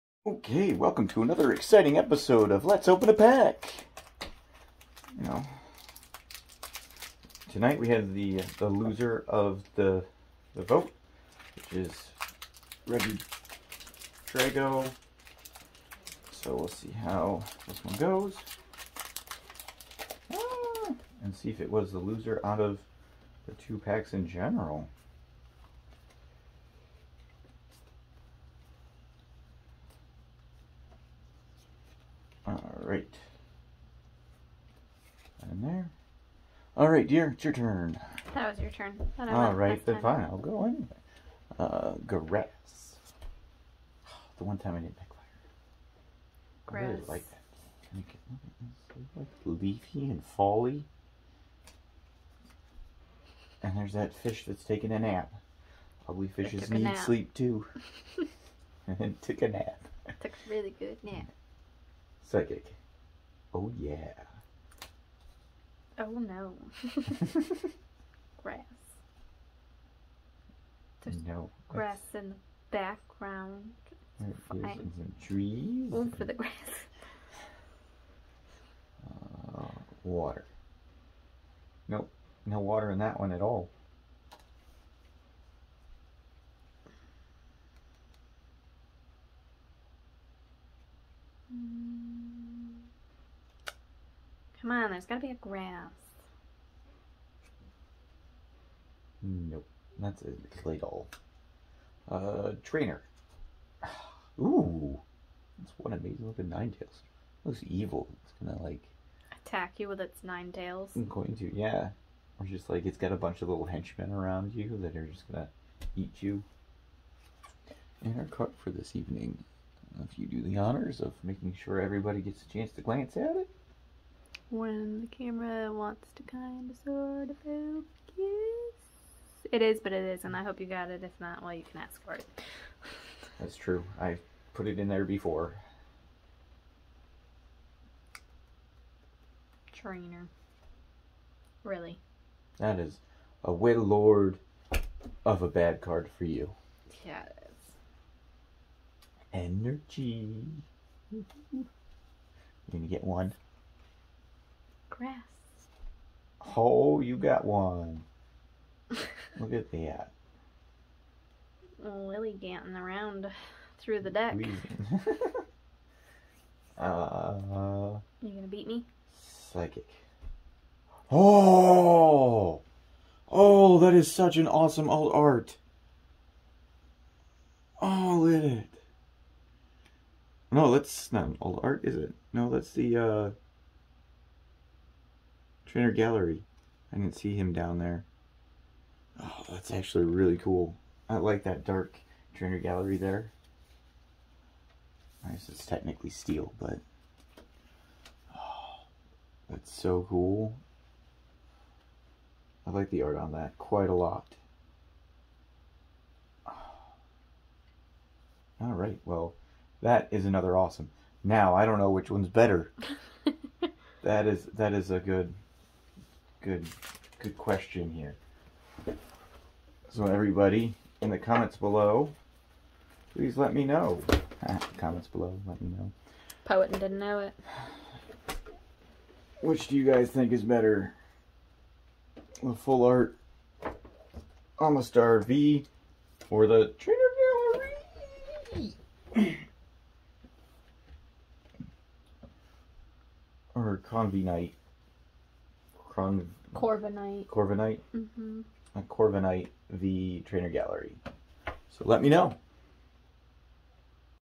Pokemon TCG: Day 175 Was the loser pack really a loser?
Tonight we opened up the losing Silver Tempest pack. I want it guessing for the first time in a long time, at least it feels that way. The night ended with an amazing pull